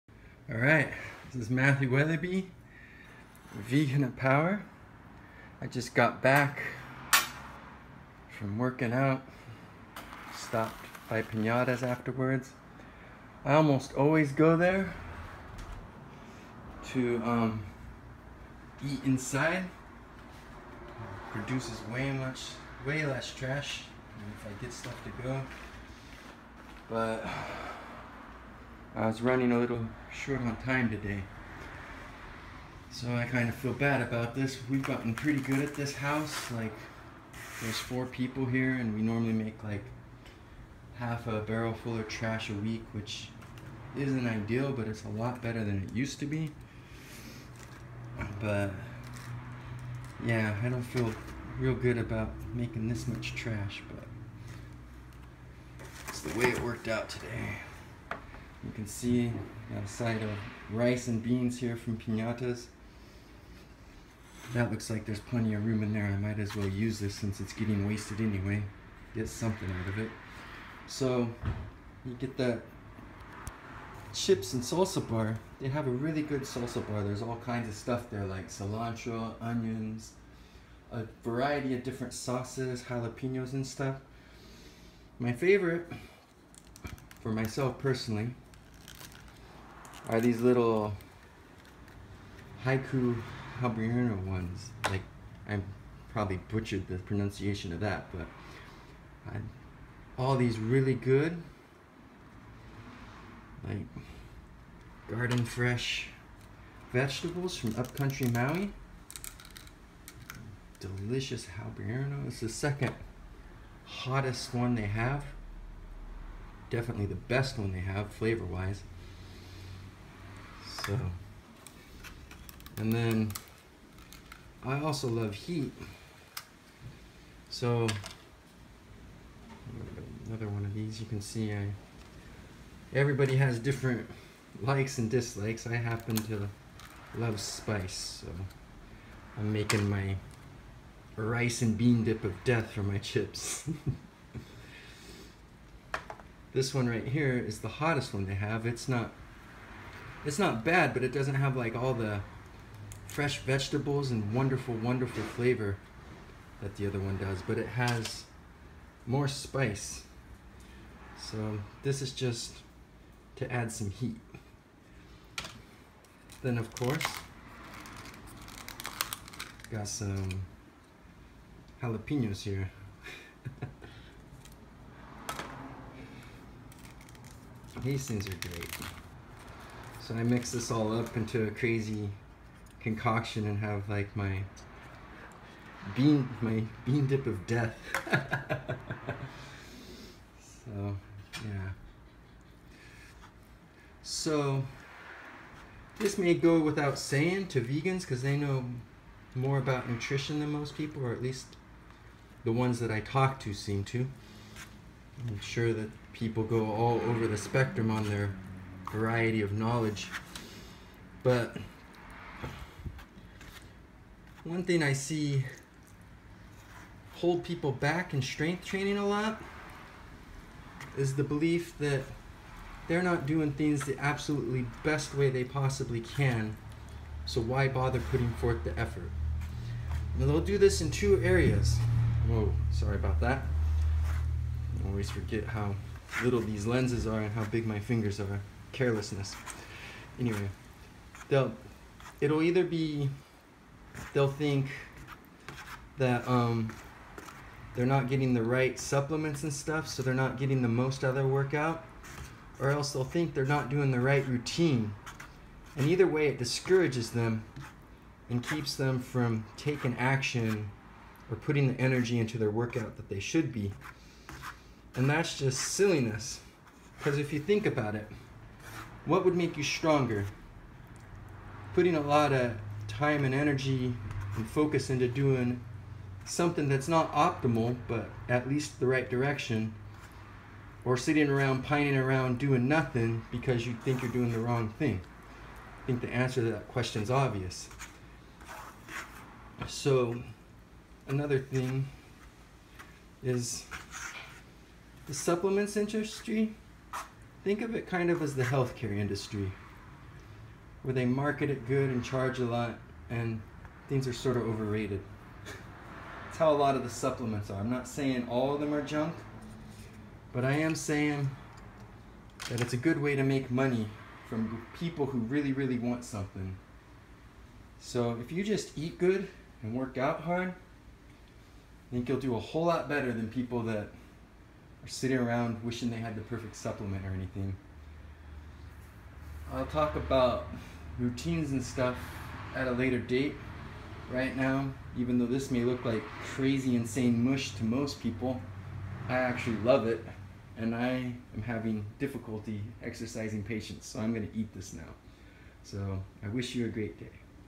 Enjoying a great meal and talking about how supplements are overrated.
I admit, me dumping all kinds of salsa and jalapenos onto my beans and rice and mixing it all together probably looks the opposite of appetizing to most of you, but man, I love this stuff.

I noticed over the years that a lot of people seem to get frozen in their progress because they think they aren't taking the right supplements or doing the right routine.

I am not saying good nutrition or smart workout programs don't make a difference.  But so long as you are getting enough rest and nutrition to recover and you are very intense and focused when you do train, that is what truly makes a difference.